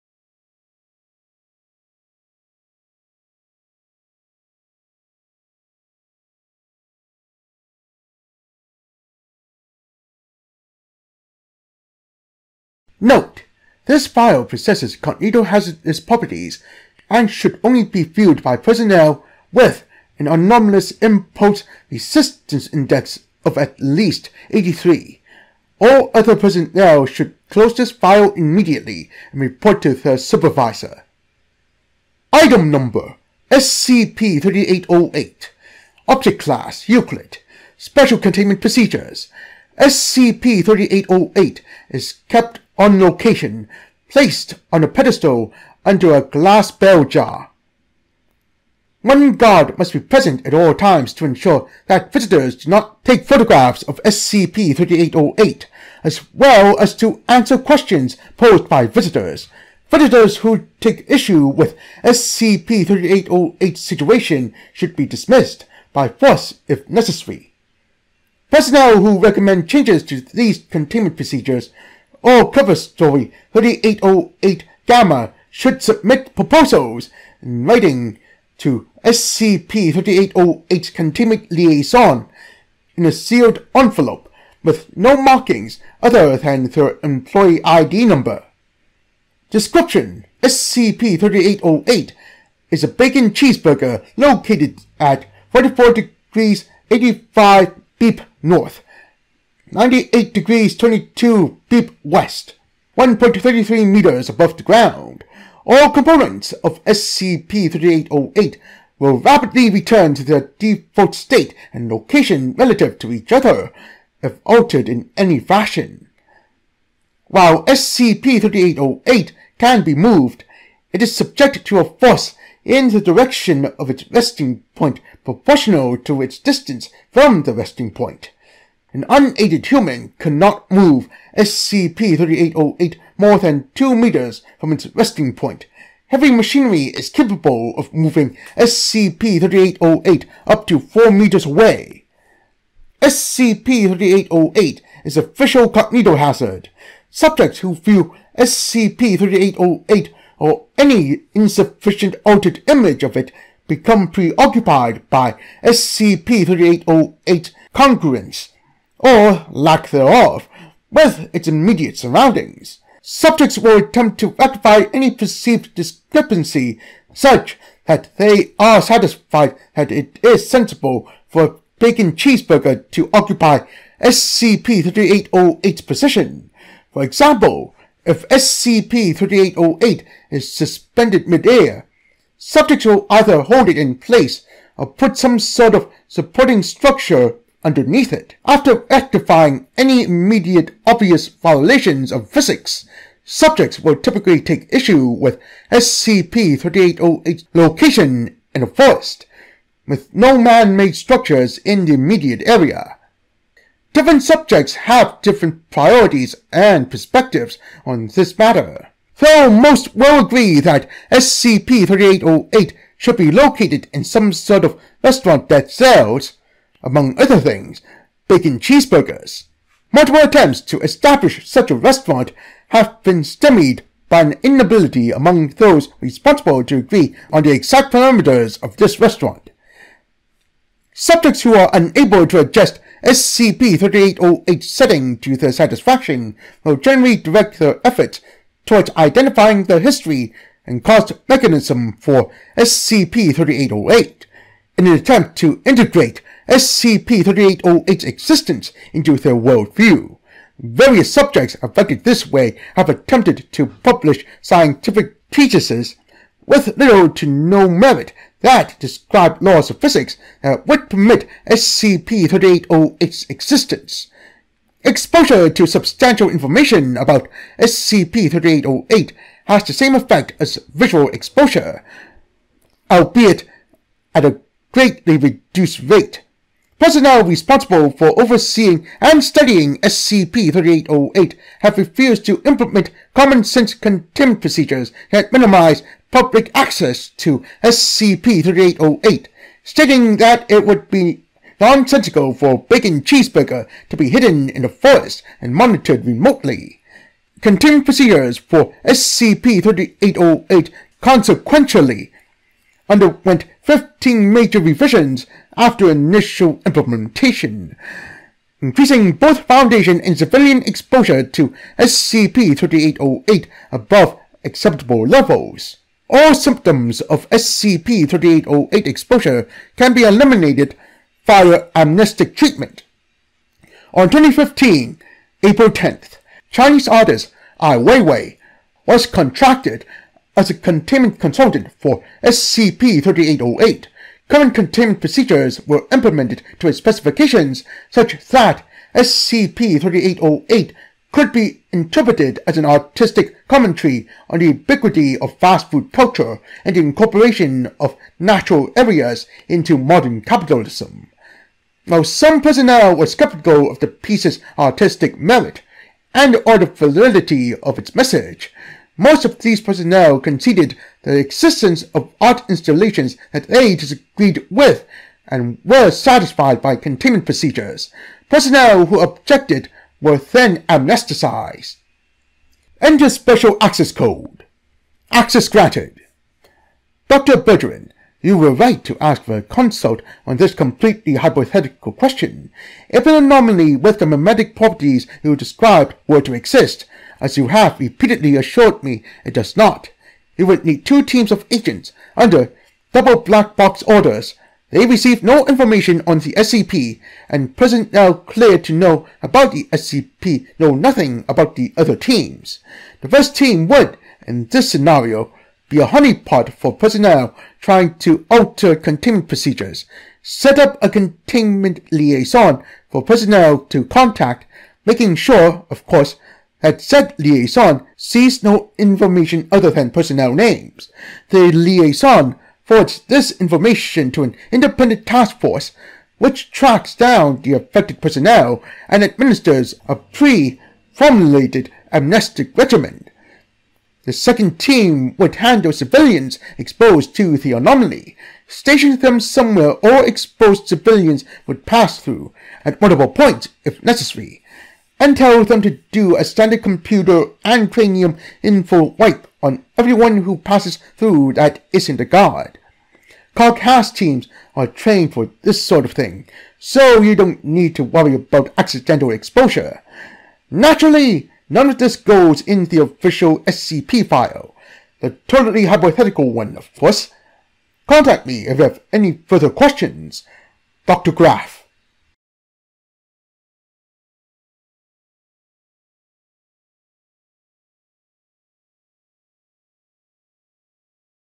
SCP 3808 Bacon Cheeseburger That Demands Justification
SCP-3808 is a floating Bacon Cheeseburger that compels you to make it more like a regular Bacon Cheeseburger.